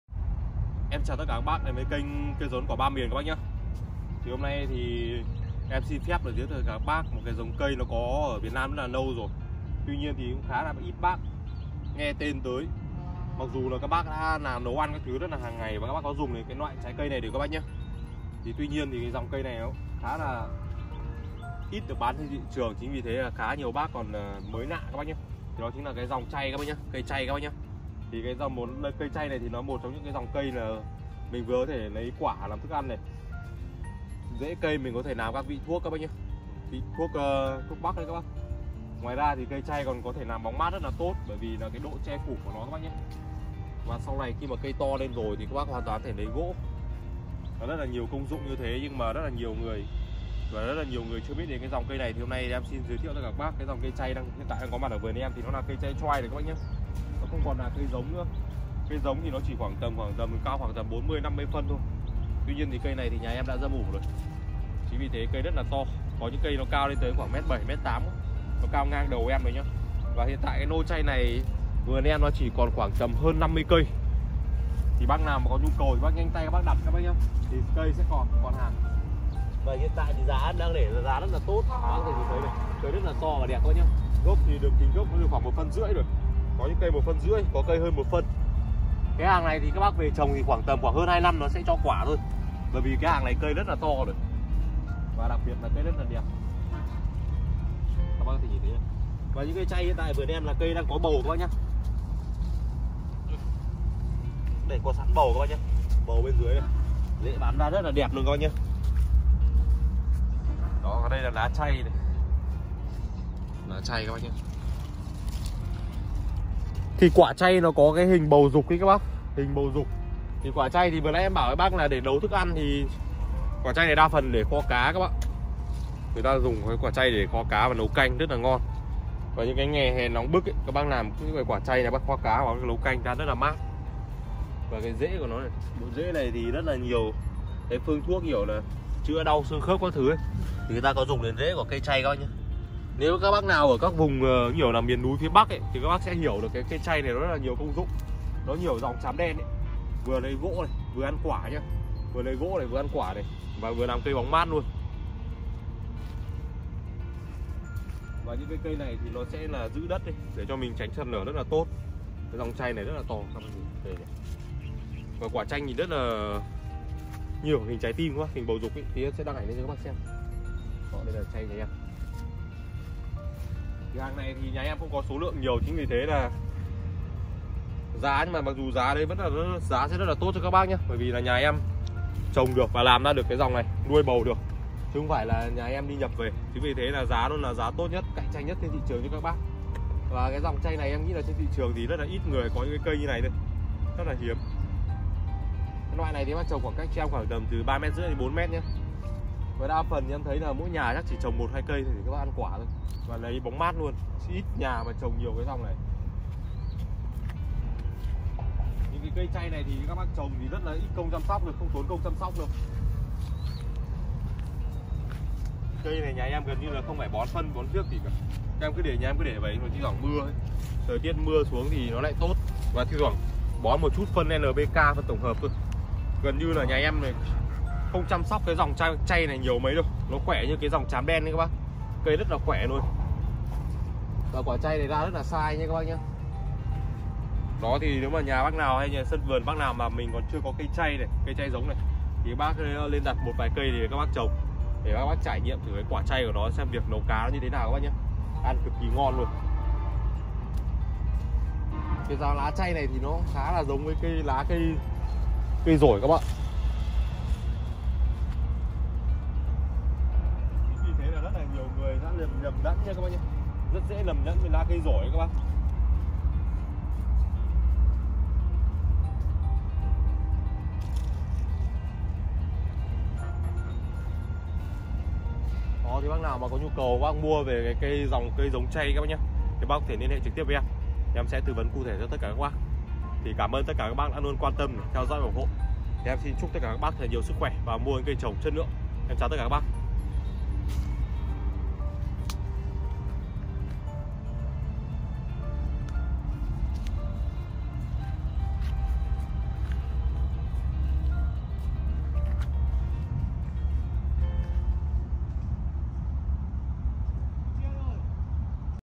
Giống cây Chay dòng cây thuần Việt với nhiều công dụng bổ ích
Giống chay là một dòng cây đặc biệt của Việt Nam vì rất ít nước có tồn tại. Chay thuộc dòng cây thân gỗ có sức sinh trưởng rất mãnh mẹ và chịu được khí hậu khắc nghiệt cũng như những loại thổ nhưỡng khác nhau. Đặc biệt dòng chay rất ít sâu bệnh hại. Cây chay là dòng cây thân gỗ chiều cao tối đa lên tới 12-15m, lá mọc so le thành hai hàng mặt nhẵn bóng phiến là hình ngọn giáo dài 20-25cm.
Cây chay cho hoa quả quanh năm đặc biệt thời điểm cho trái năng suất nhất từ tháng 6-8. Quả chay có rất nhiều công dụng như kho cá, nấu canh, dễ chay có thể làm được liệu hoặc dùng để ăn trầu từ xa xưa ông cha ta vẫn sử dụng và đó nó sự phát hiện khởi nguồn của giống chay
Thời điểm đề trồng cây chay tốt nhất là vào mùa mưa lúc lập xuân hoặc tháng 7,8
SDT TƯ VẤN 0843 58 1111_ZALO